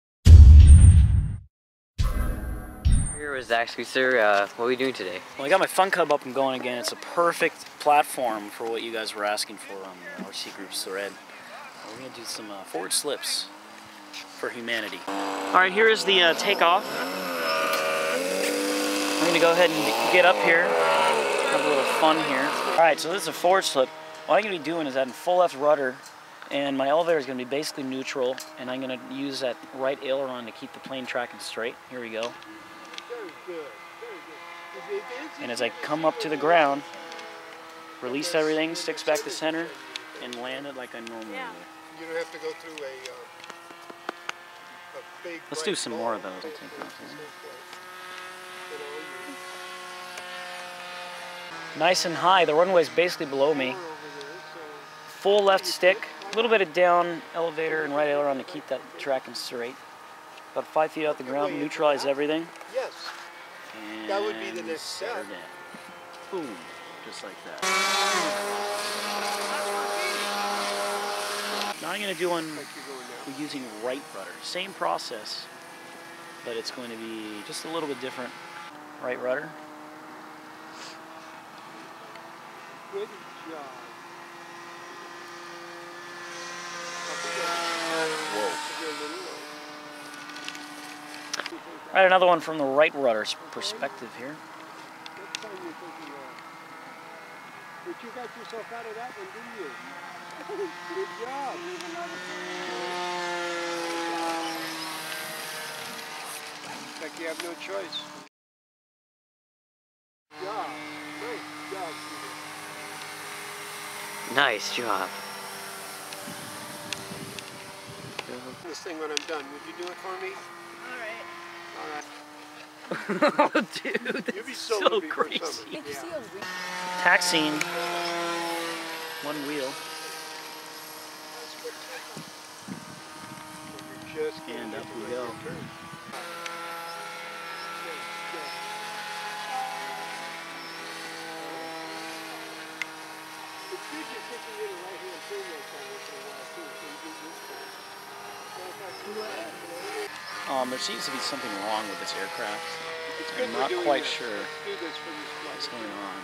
Forward Slip Tutorial for R/C Airplanes
Forward Slip Tutorial for R/C Airplanes, along with some fun Funcub flying at the end as well.

Forward Slip Official Definition:

The forward slip will change the heading of the aircraft away from the down wing, while retaining the original track (flight path over the ground) of the aircraft. A forward-slip is useful when a pilot has set up for a landing approach with excessive height or must descend steeply beyond a tree line to touchdown near the start of a short runway. Assuming that the runway is properly lined up, the forward slip will allow the aircraft track to be maintained while steepening the descent without adding excessive airspeed. Since the heading is not aligned with the runway, the slip must be removed before touchdown to avoid excessive side loading on the landing gear, and if a cross wind is present an appropriate side slip may be necessary at touchdown as described below.